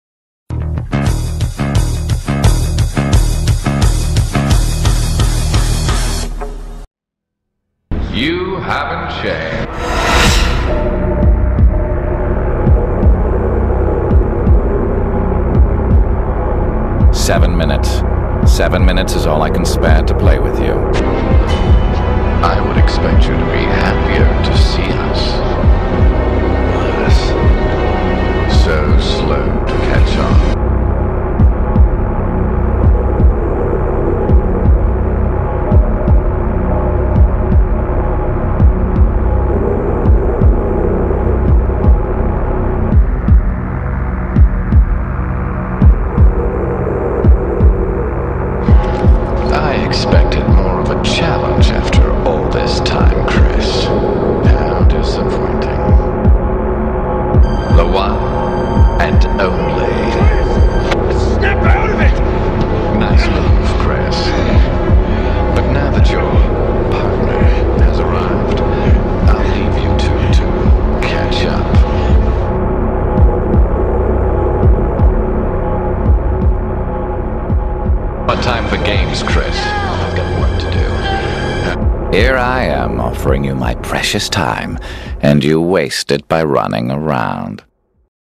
ONE-MAN-DESTINY(S.S.S) BEAT Hugoboss92320&Albert-WeskerMTA(DEAD X)
You Remember Hugo (You Beat Me) Cause I Played Dconfiguration,After You Happy Win Face My Brother With A Fake ID (IP---MAN) BASTARD Anyway The Fact Is YOU NEVER BEAT ME WITH Bconfinguration So Who I See I Never Lose Face you ME 2xTimes For NOW I See 2--O I BEAT YOU HUGOBITCH AND Albert-WeskerMTA,My Partner Is Not Of SSS Team,Anyway I Promise You HugoSMD It s Only Just Begun